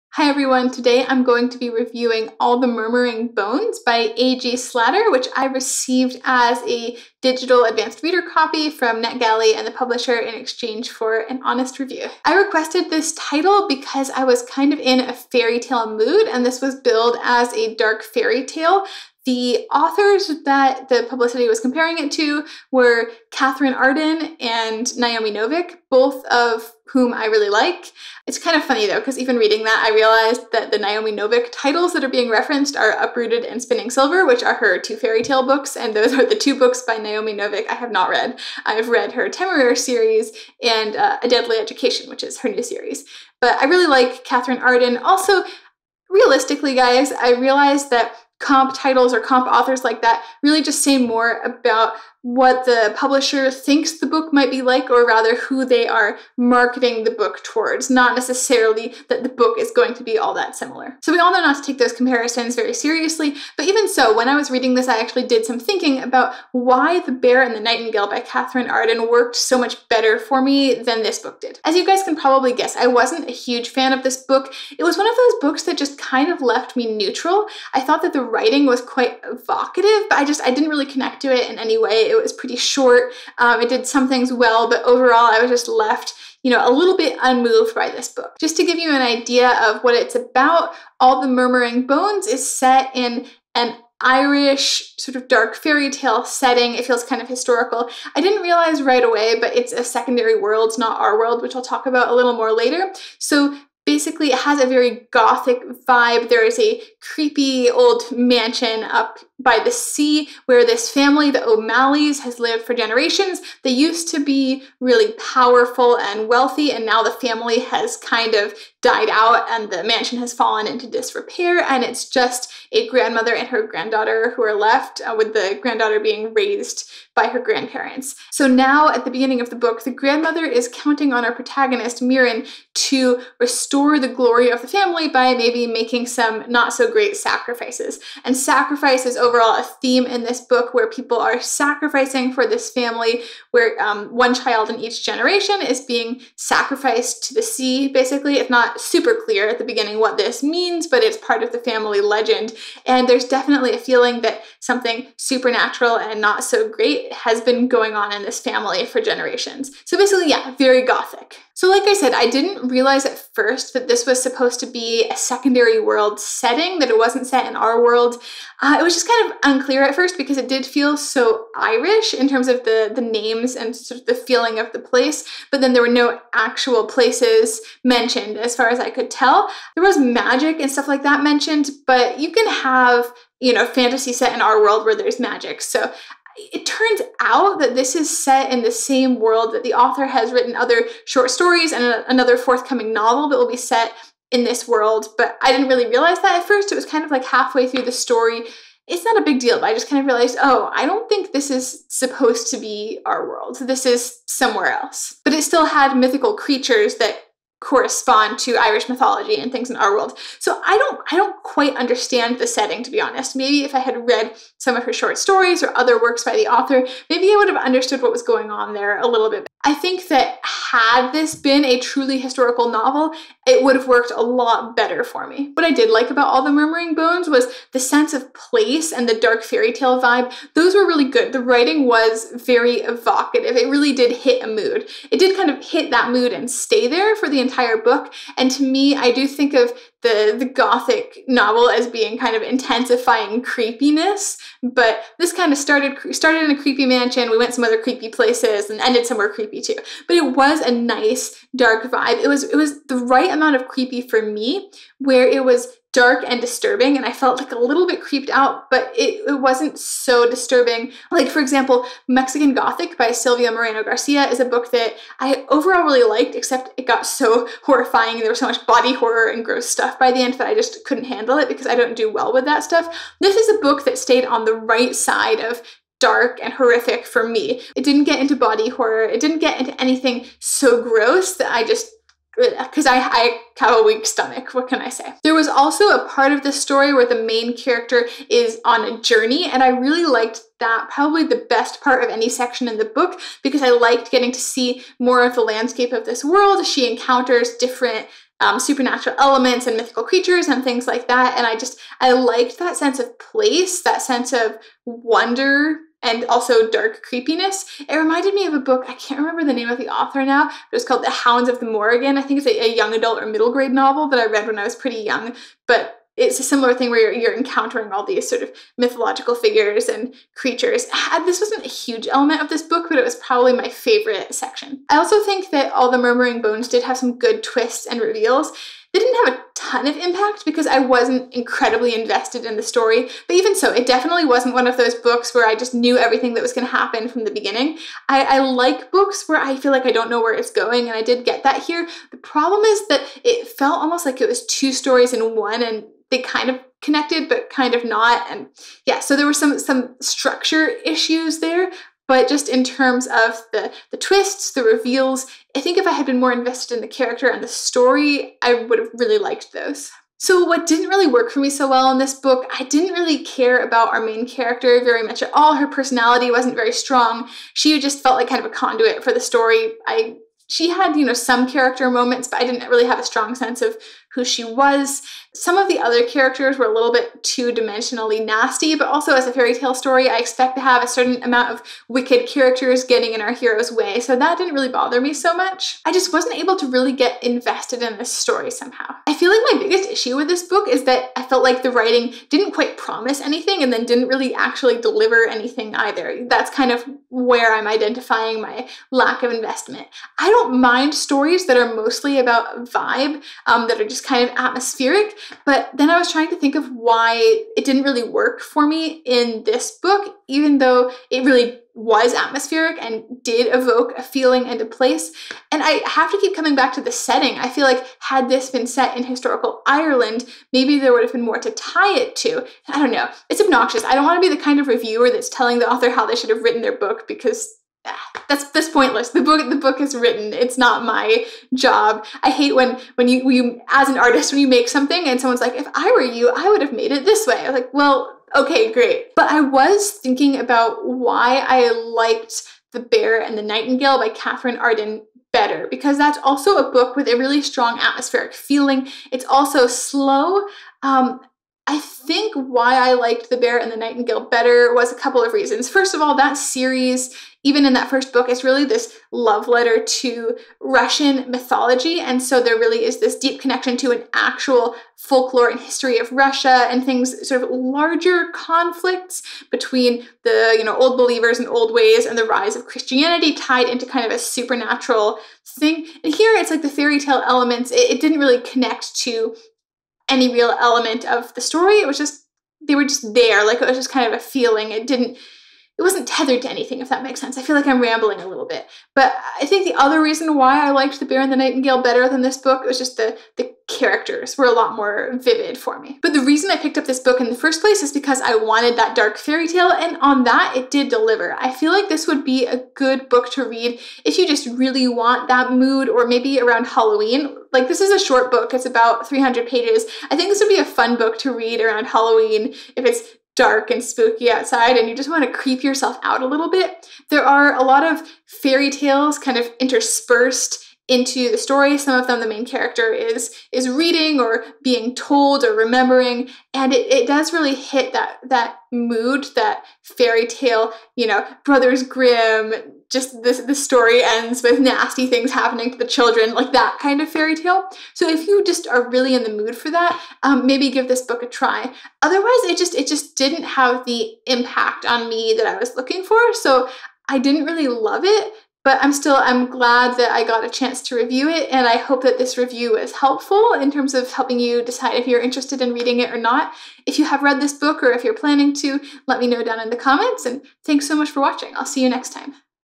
All the Murmuring Bones by A.G Slatter | Spoiler-free Review
I didn't love this dark fairytale, but it did evoke a pretty strong gothic mood. My review of All the Murmuring Bones by A.G (Angela) Slatter, which released on March 9, 2021.